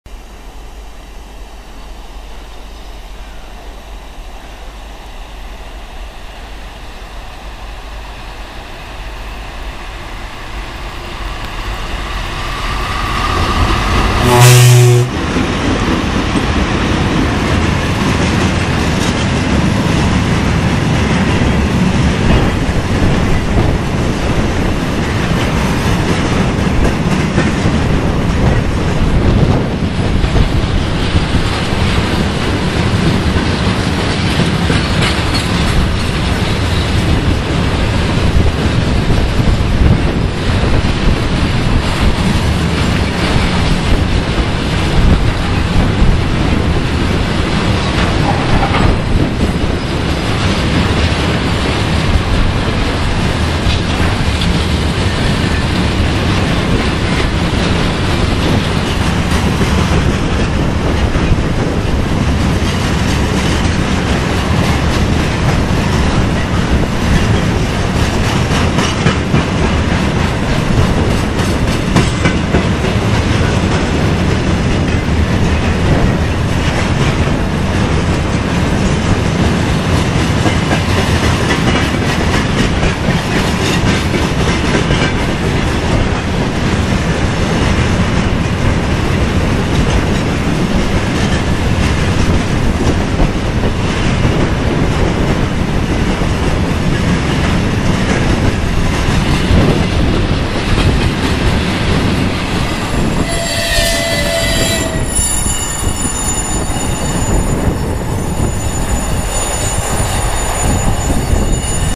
ВЛ80К-496 с грузовым поездом, МПТ4-562
Электровоз ВЛ80К-496 с грузовым поездом и мотовоз МПТ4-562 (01:43), перегон Развилка - Кизитеринка, участок о.п. Красный Аксай - ст. Кизитеринка, г. Ростов-на-Дону, Северо-Кавказская железная дорога, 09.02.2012 г.